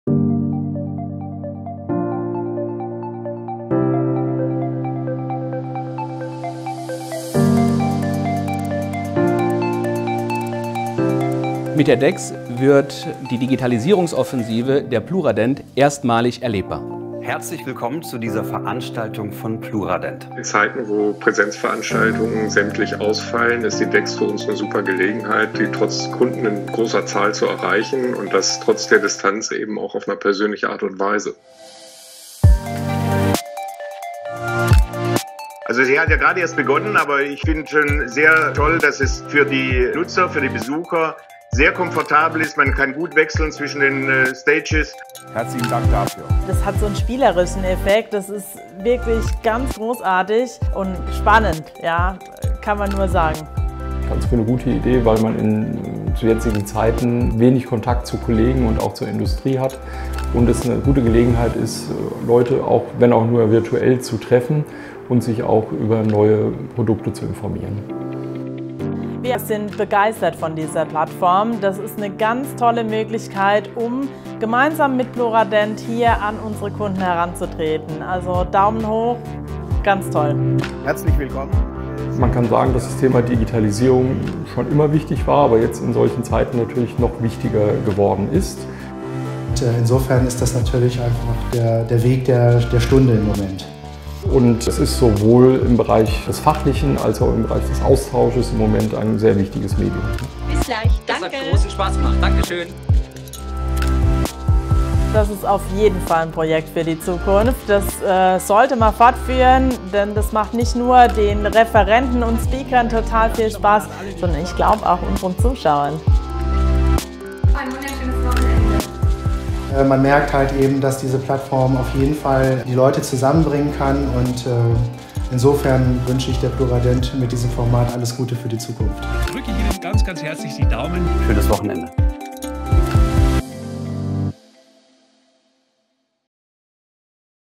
D:EX Dental Experience by Pluradent
Über 1.200 Zahnärzte, Zahntechniker und Partner nahmen am 24. April 2021 an der D:EX Dental Experience teil. Neben Produktneuheiten stellten Experten auf der neuen, interaktiven Plattform Praxisbeispiele und Trends in den Bereichen digitale Transformation, Praxismanagement und Hygiene live vor.